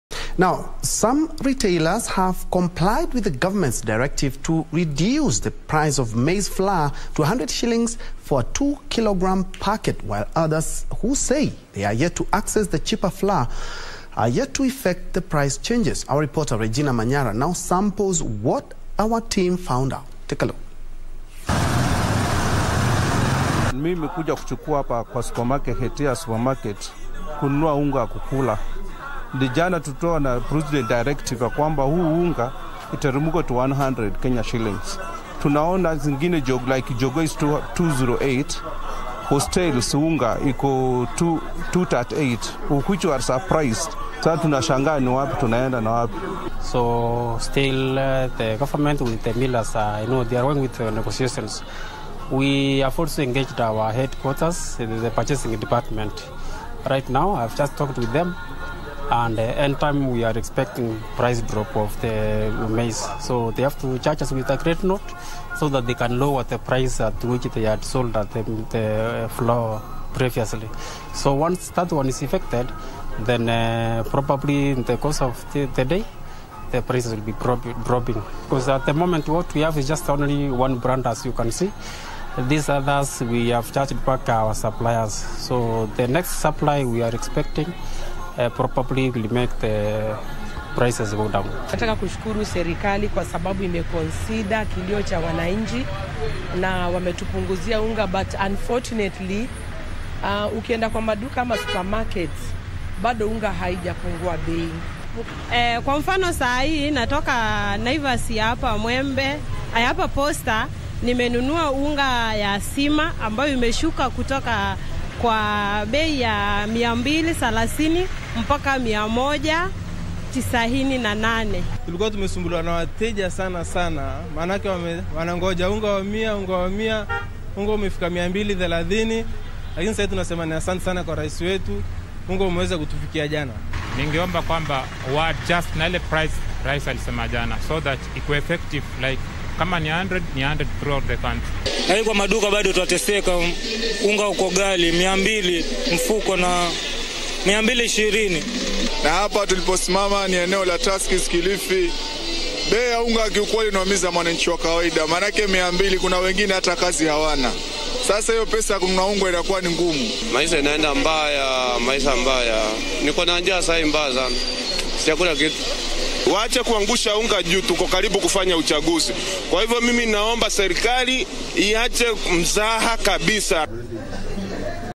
Retailers review downward maize flour prices
Some retailers have complied with the government’s directive to reduce the price of maize flour to 100 shillings for a two-kilo packet while others who say they are yet to access the cheaper flour, are yet to effect the price changes. Our reporter Regina Manyara now samples what our team found out.